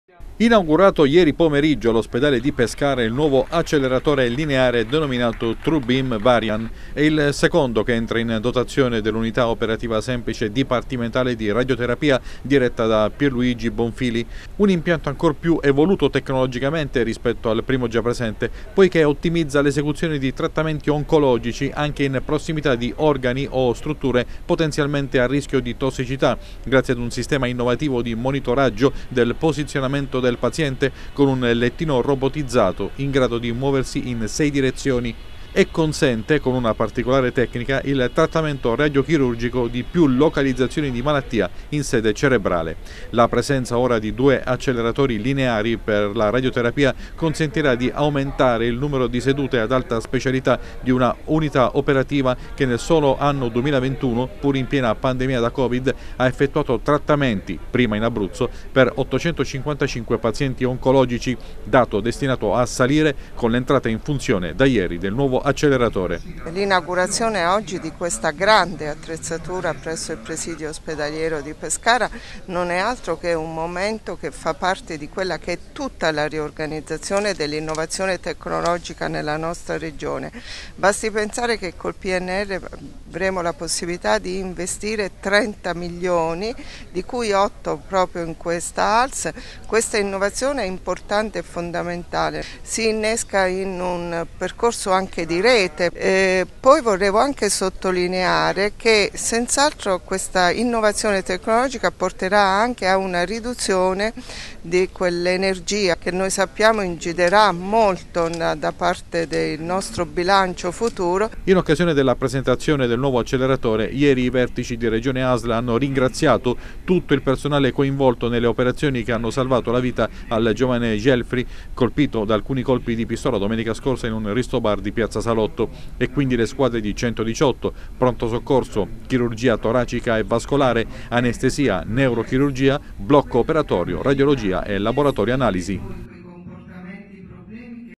Asl Pescara. Entra in funzione il nuovo acceleratore lineare
E' entrato in funzione il nuovo acceleratore lineare nell'unità di Radioterapia della Asl di Pescara. Si tratta di un secondo impianto, ancora più sofisticato del primo, destinato a potenziare i trattamenti oncologici effettuati dall'azienda sanitaria